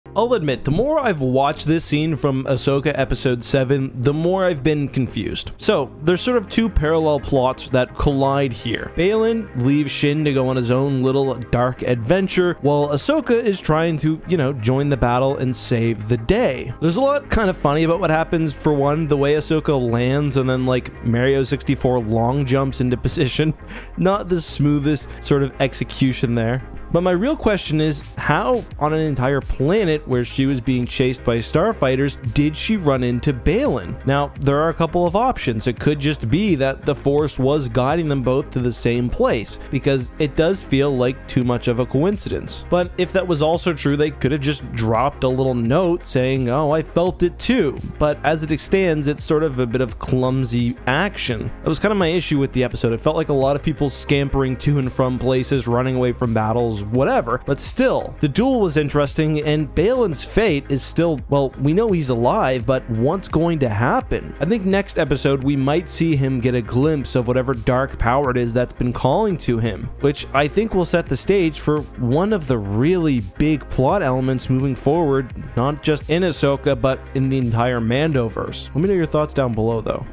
This was REALLY weird...
I think episode 7 of Ahsoka was missing a crucial detail and led to something very inconsistent, when Baylan Skoll and Ahsoka ended up almost randomly running into eachother.